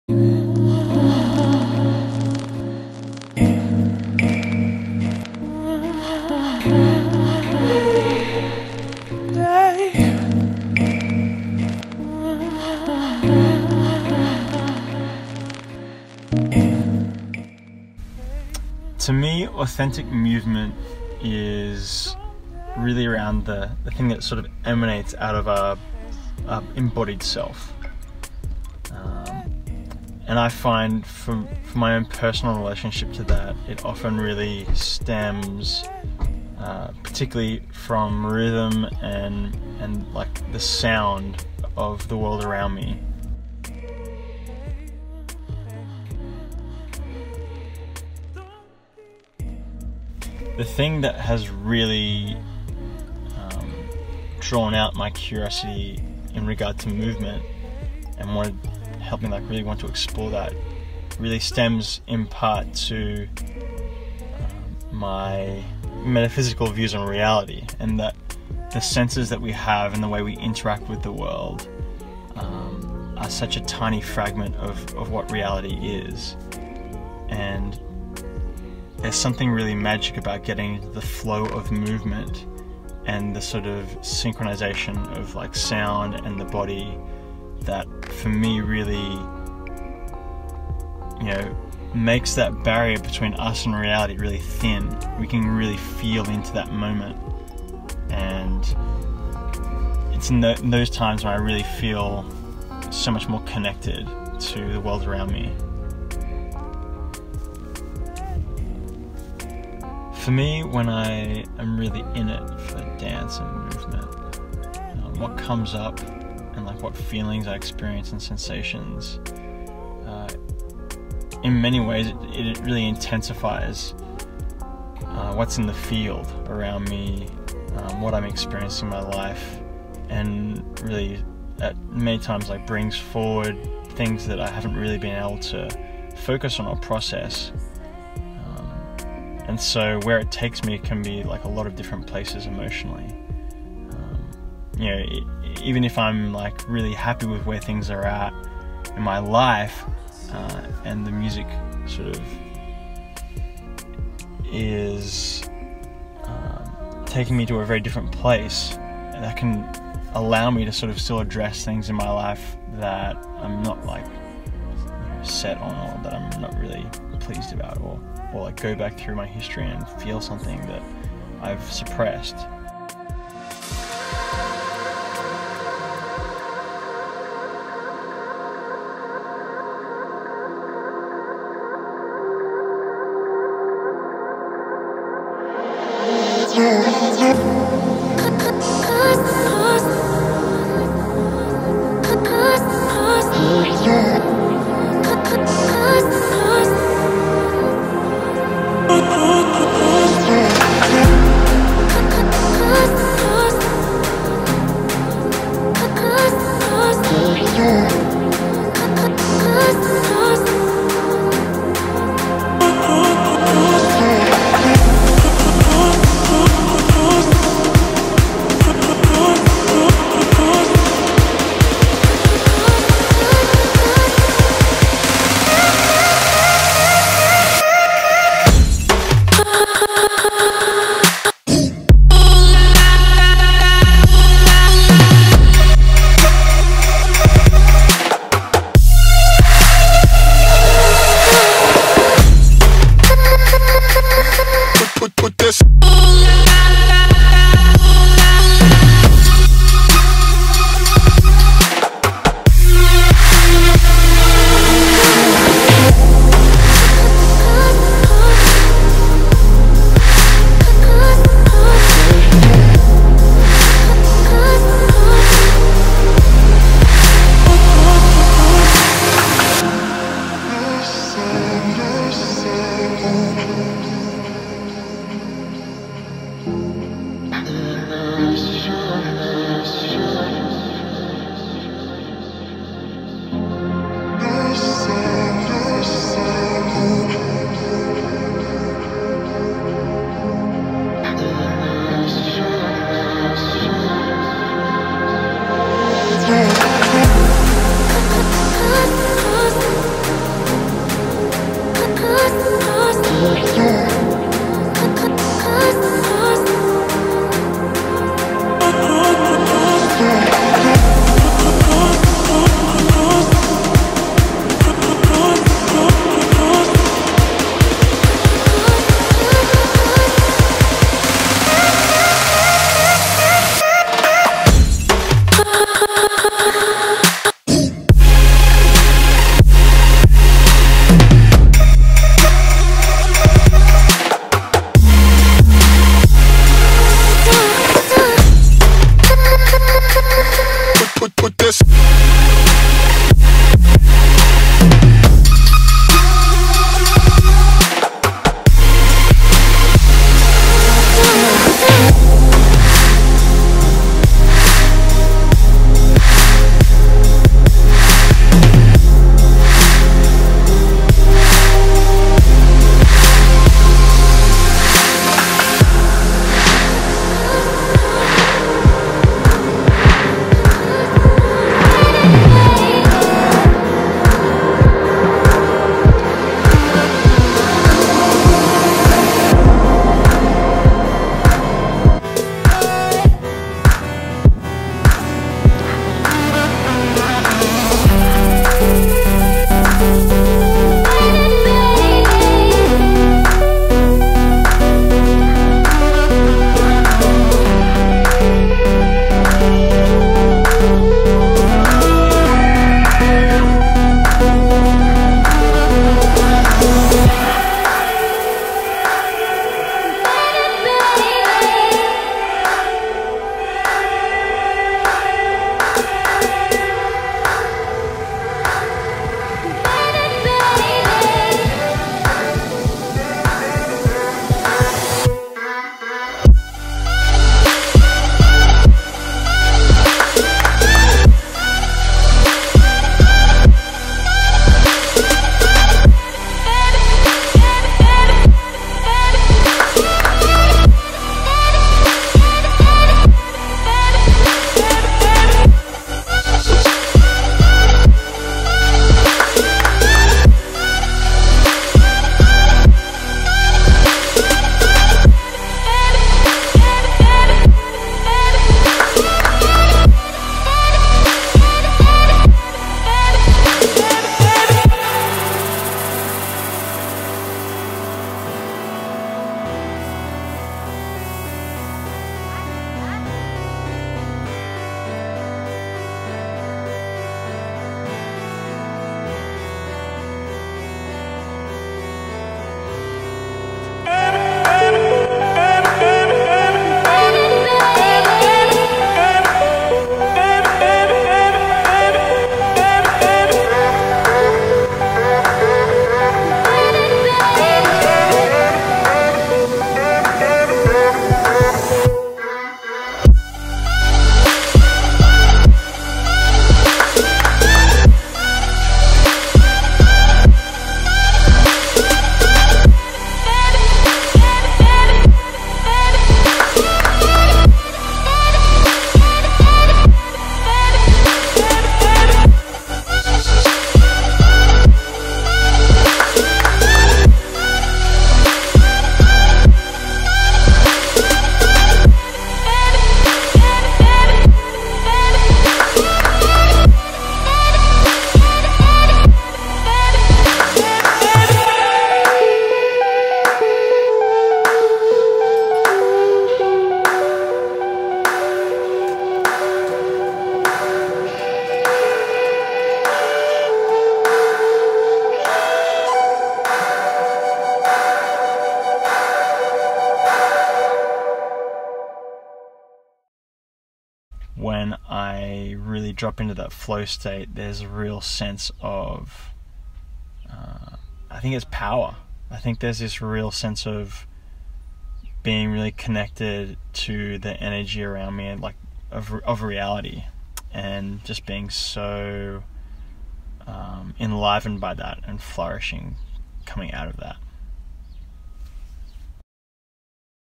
Vlog Series- Authentic Movement With Tom Watson
Subject: Tom Watson-
Tom Watson is a self taught movement artist living in the Bay Area, CA.

Filmed& Edited By: Jojo Lamboy